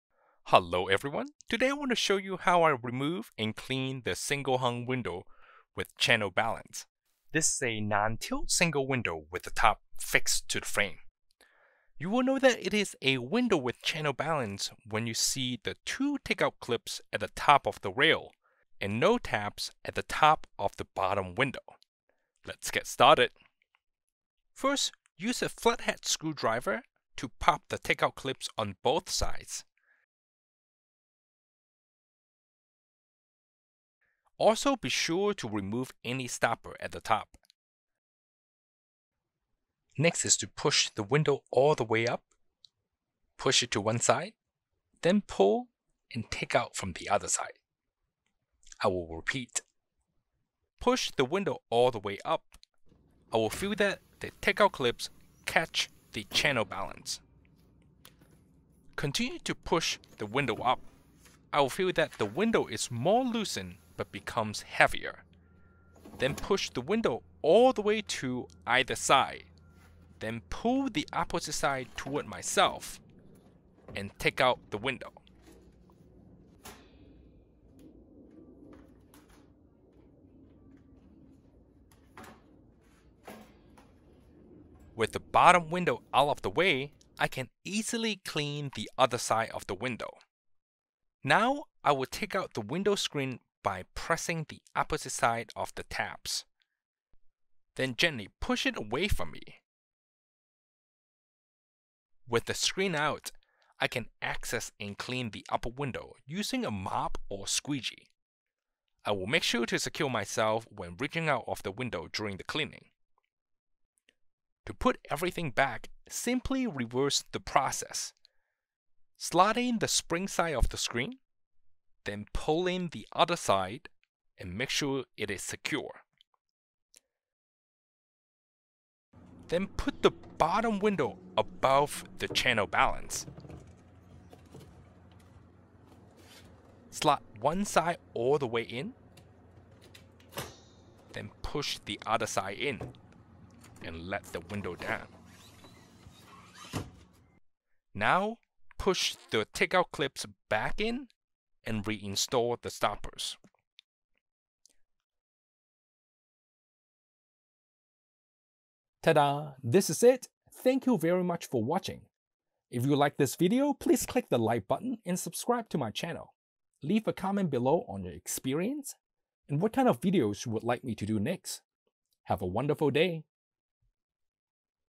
How to: Remove: Clean: non-tilt Single-Hung Window with Channel Balance
This video shows how to remove the single hung window with channel balance (non-tilt) and window screen to access the outside.

Safety first. Be sure to secure oneself and use a squeegee when cleaning.

I am not liable for any PERSONAL INJURY, PROPERTY DAMAGE OR WRONGFUL DEATH caused by this video.